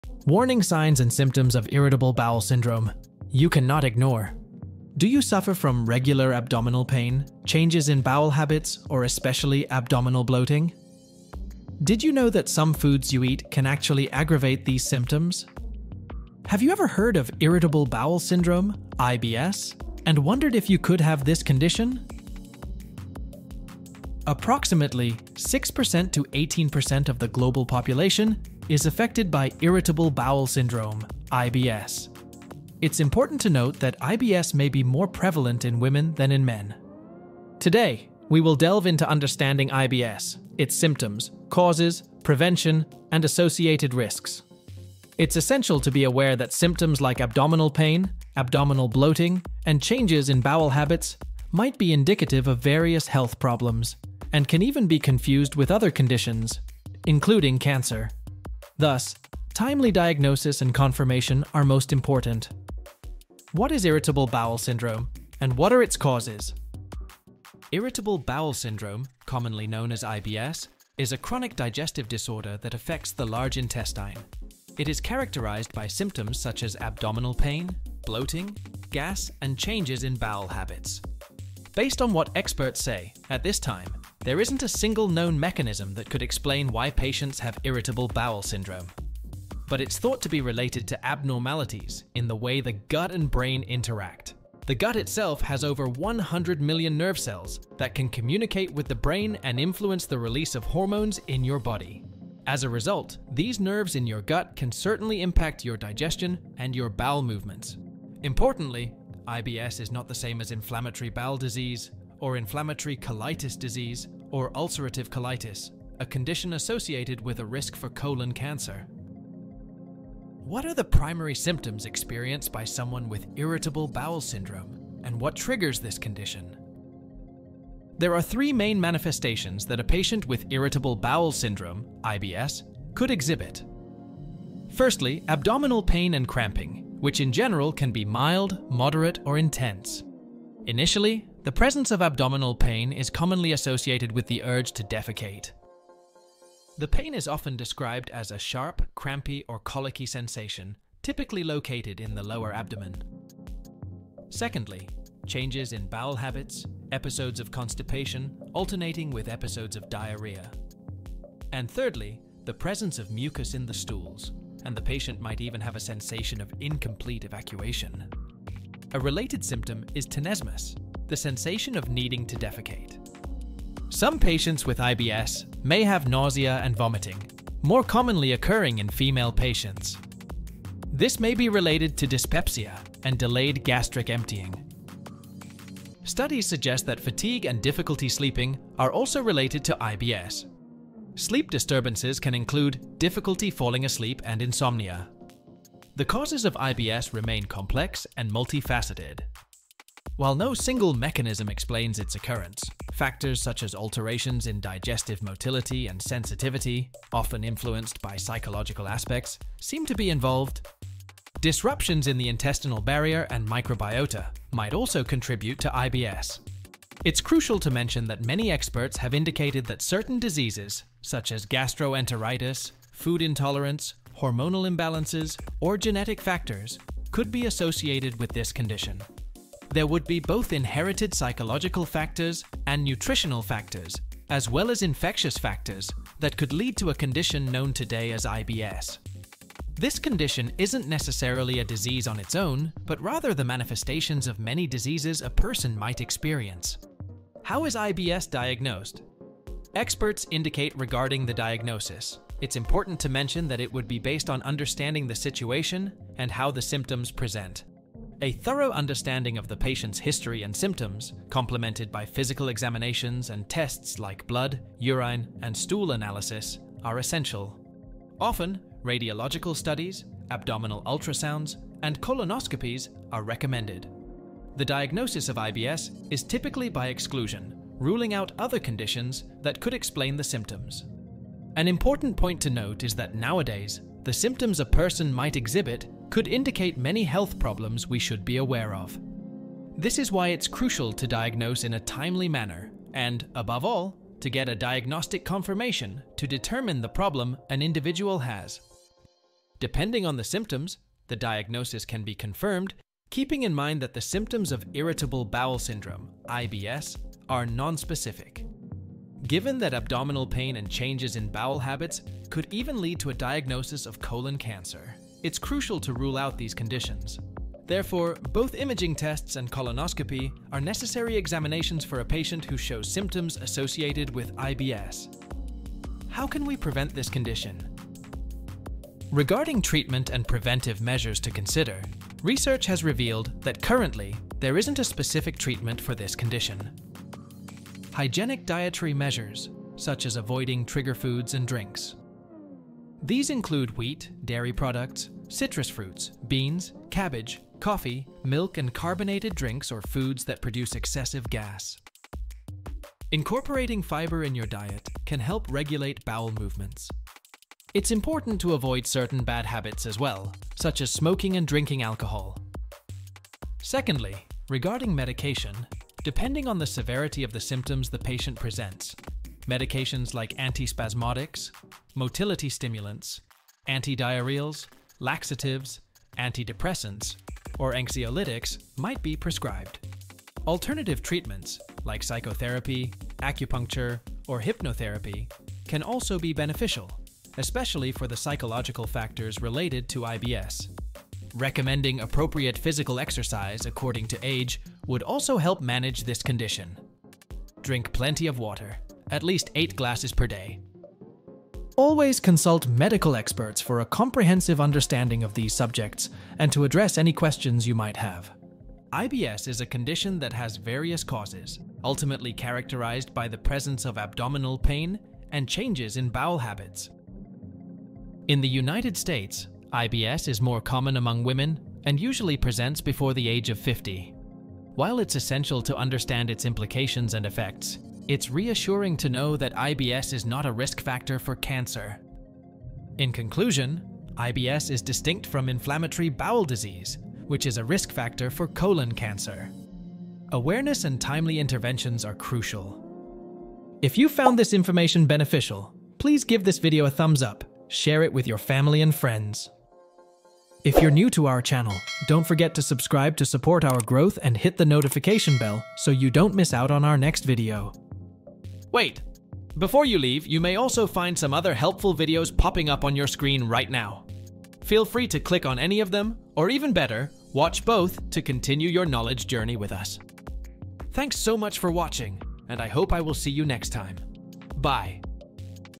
Warning Symptoms of Irritable Bowel Syndrome(IBS) You Can't Ignore!
Are you frequently experiencing digestive issues but haven't considered the possibility of Irritable Bowel Syndrome (IBS)? Tired of running to the bathroom and never feeling relief? You may have IBS, but you are not alone.

Irritable Bowel Syndrome, or IBS, is a condition that affects a significant portion of the global population, causing symptoms like abdominal pain and changes in bowel habits. Surprisingly, IBS isn't just an ailment for the food intolerant or those with bad eating habits. Modern investigations have also linked it to psychological factors and even genetic predispositions.

Alarmingly, IBS isn't just about an upset stomach anymore. Recent research shows it's connected to a complex interplay between the gut, brain, nerves, and hormones.

In this video, we'll delve into understanding IBS - its symptoms, causes, prevention, and associated risks. Recognizing IBS warning signs and getting properly diagnosed is crucial for managing this chronic condition.

IBS is characterized by chronic symptoms like abdominal pain, bloating, and bowel habit changes. But what exactly causes this complex disorder, and are there any risks?

By understanding IBS triggers, available treatments, and key differences from other conditions, you can take back control. Let's explore the ins and outs of this chronic disorder so you can get the relief you need. Ready for some solutions?

Don't let IBS control you - learn to control it! This article will uncover everything you need to know about living well with IBS.

Prepared to discover the signs, symptoms, and risks of irritable bowel syndrome? Stick with us to uncover the causes, symptoms, diagnosis, and management of IBS. By educating yourself, you can ease symptoms and start feeling better. Make sure to watch until the very end.

Don't miss out on this essential information. Your health is in your hands; stay informed!

⏰TIMESTAMPS:

00:00 - Intro
01:05 - What is Irritable Bowel Syndrome? And What are its Causes?
02:02 - Primary Symptoms of Irritable Syndrome (IBS)
02:06 - What Triggers This Condition?
04:07 - How is IBS Diagnosed?
05:28 - How Can We Prevent IBS?

IBS symptoms
Abdominal Pain
Constipation